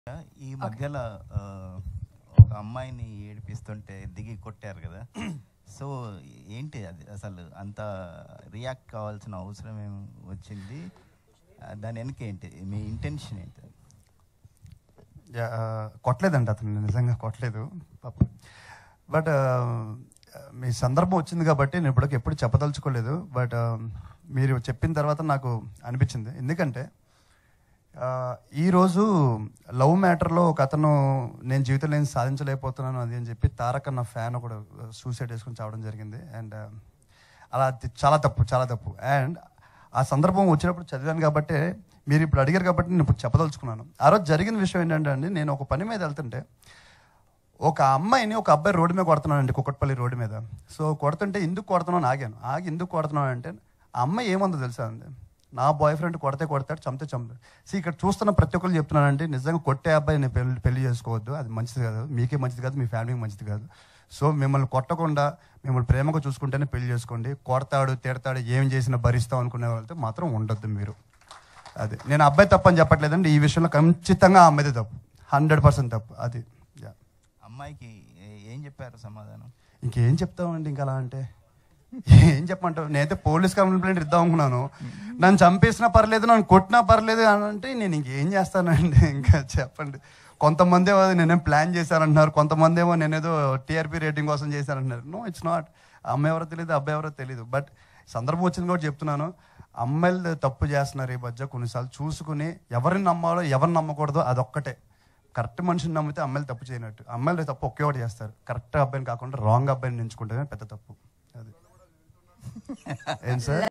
ఆరోజు వాడిని రోడ్ మీద అందుకే కొట్టాను || Naga Shaurya About Roadside Lovers Fight Incident || NS
Watch Here NagaShaurya About Roadside Lovers Fight Incident